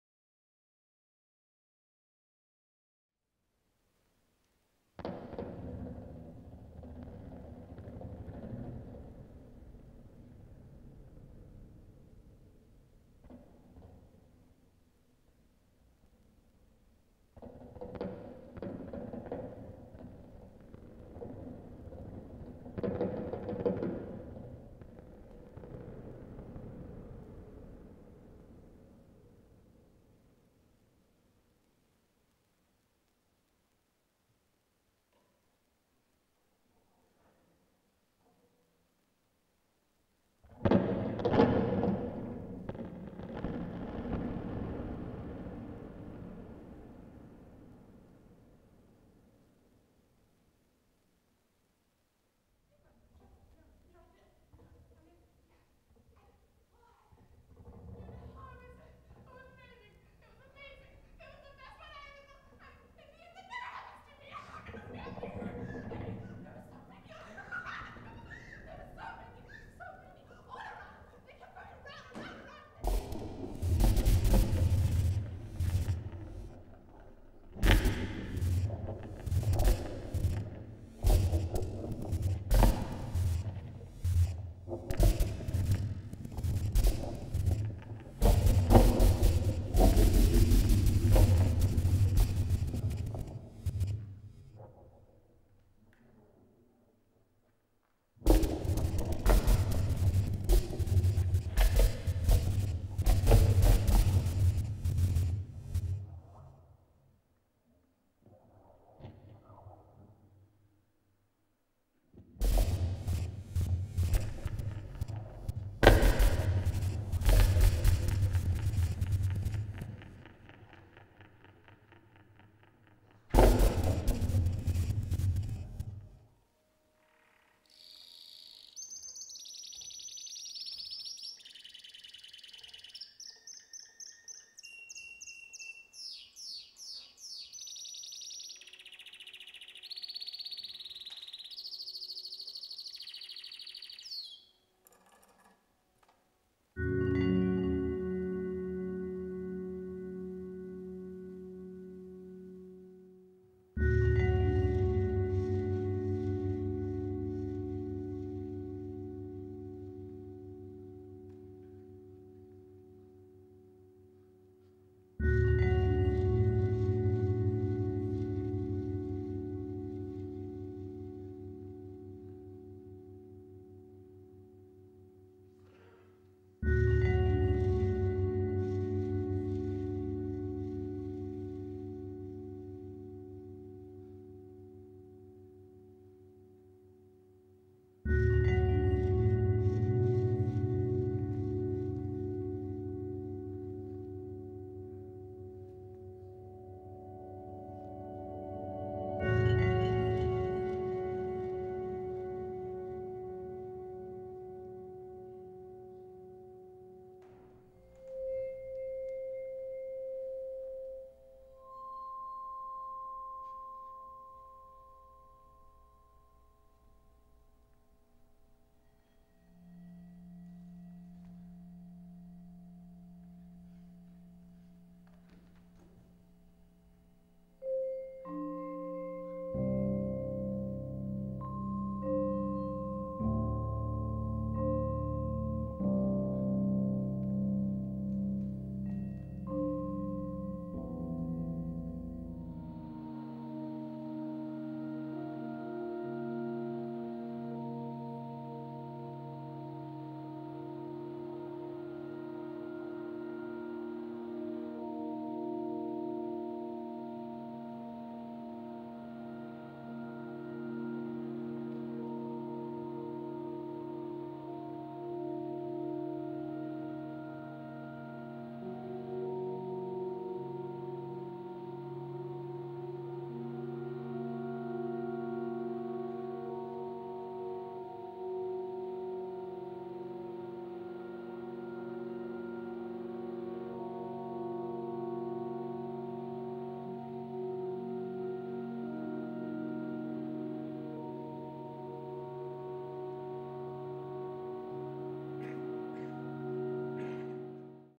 Evergreen by Fernando Hernando Magadan
Choreography for 6 dancers for which I created an interactive audioinstalation consisting of sensor equipped bungee cords and electronics. Premiered in May 2008 in Den Haag / the Netherlands.